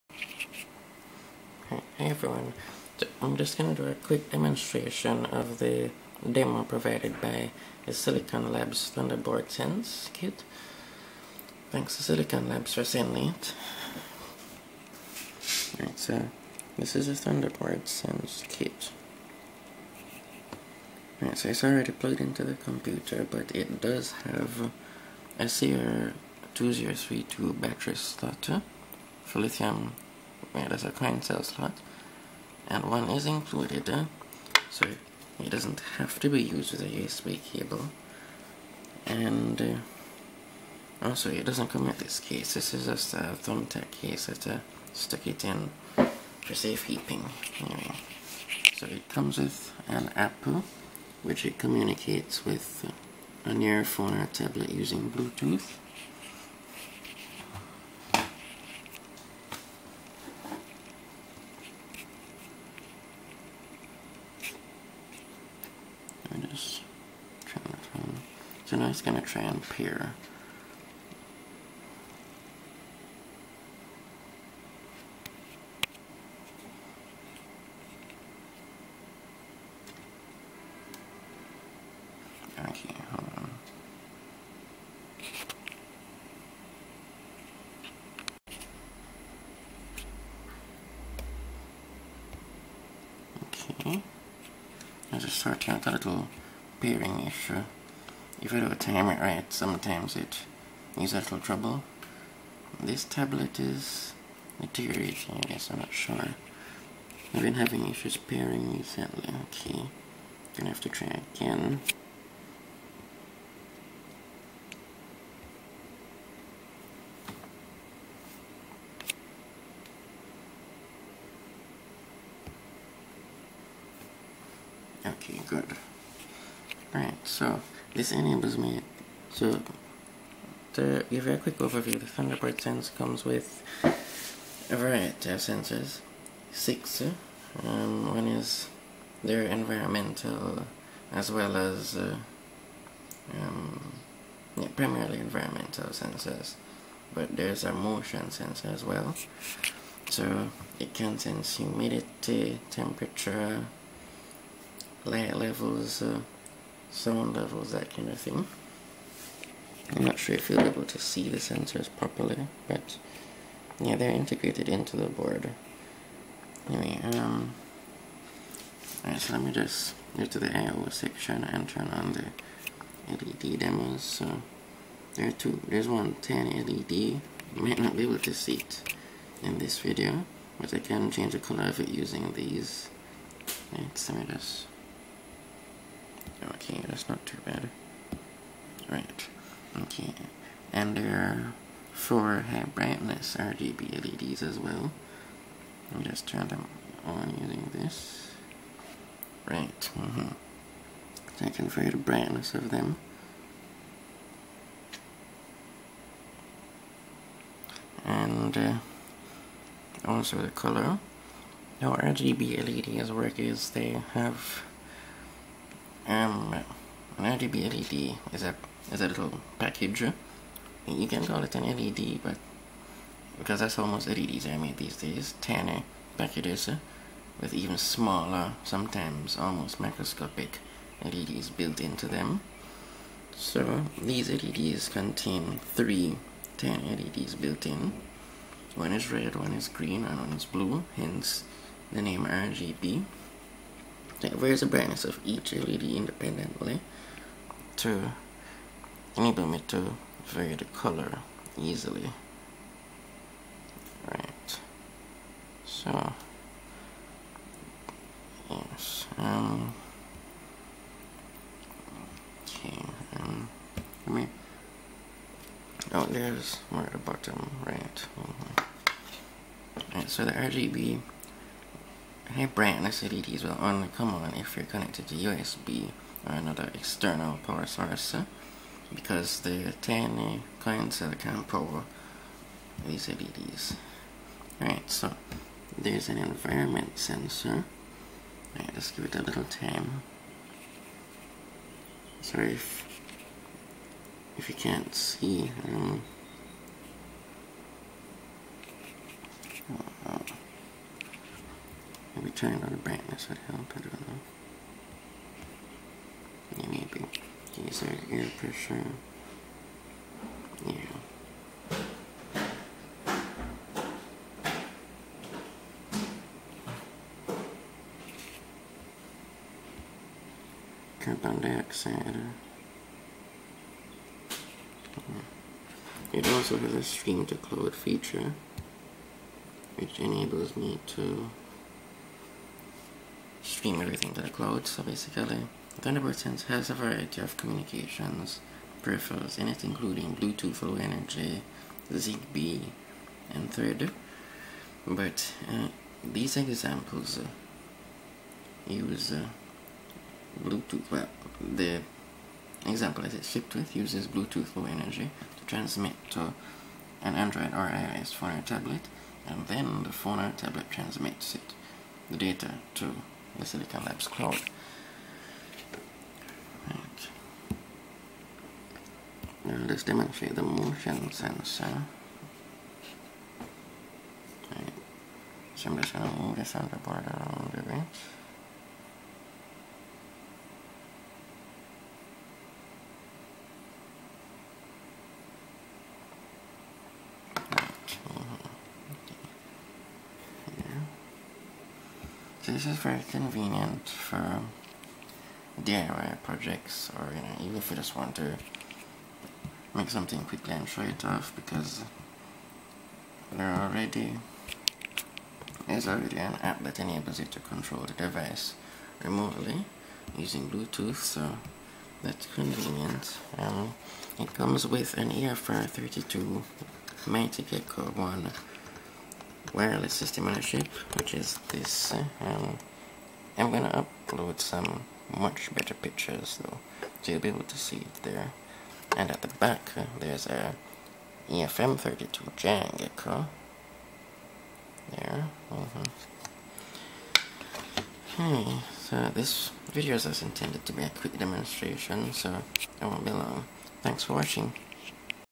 Thunderboard Sense Wireless IoT Kit - A Demo
In this video we take a look at the Silicon Labs Thunderboard Sense IoT kit with a wireless system on a chip (SoC) that supports Bluetooth Low Energy (BLE), ZigBee (802.15.4), and Thread.